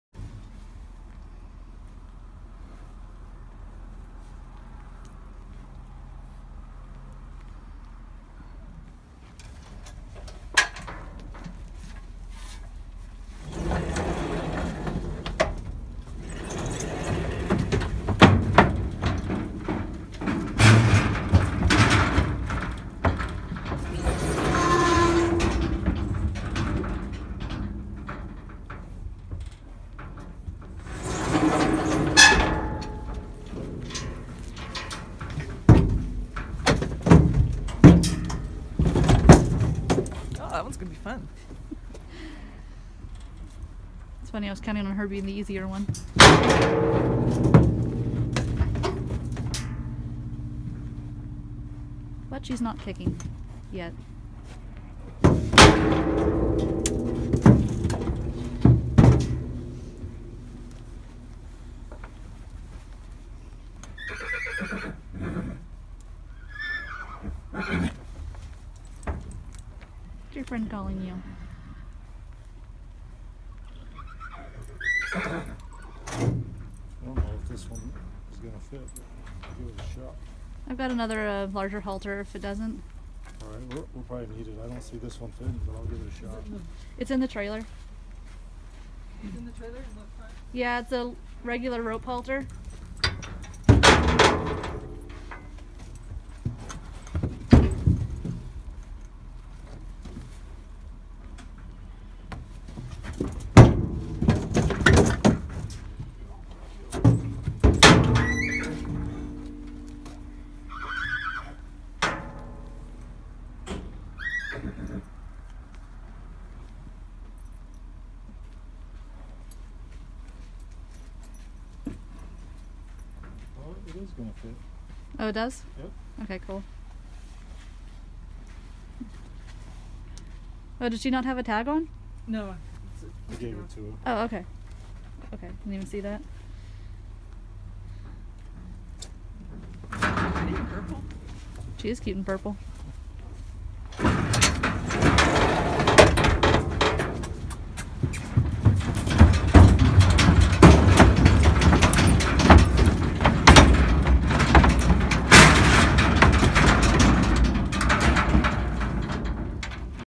April 6, 2018
Loading Shadow and Aayla from palomino valley back in January.